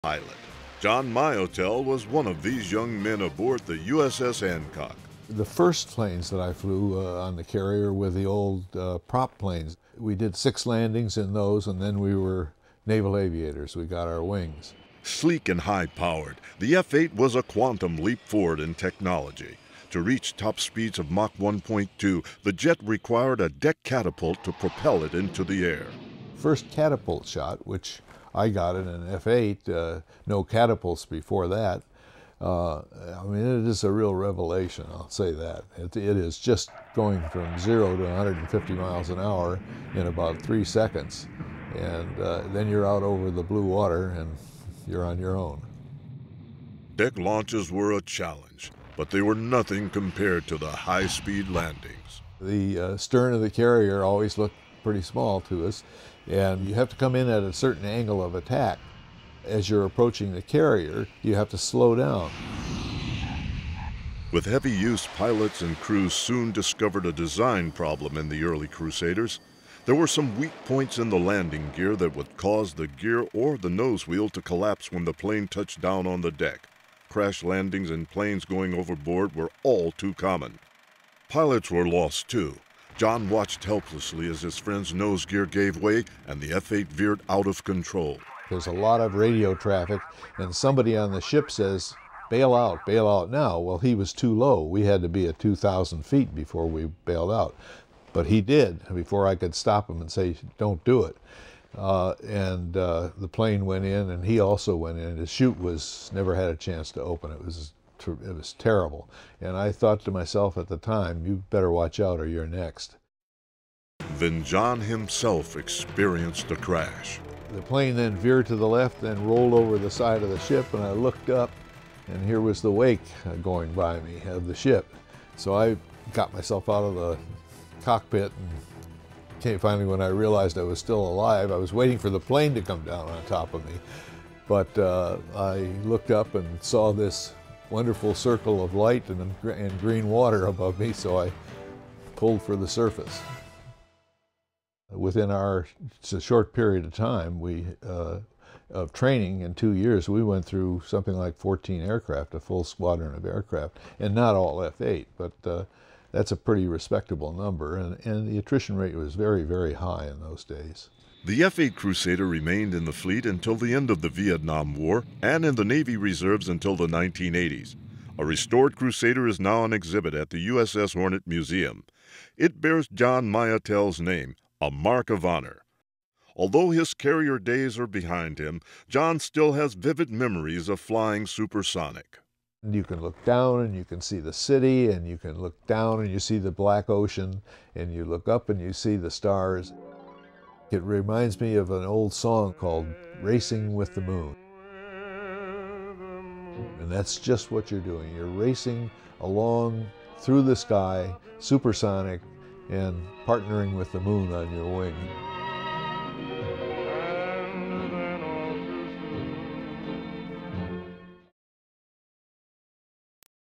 Share Your Story: John Miotell
John Miottel was one of the first Navy pilots to fly the F-8 Crusader. Flying at supersonic speeds, this new breed of pilot had to train himself to deal with such new and precarious technology, the experience of which has often been compared to "racing with the moon".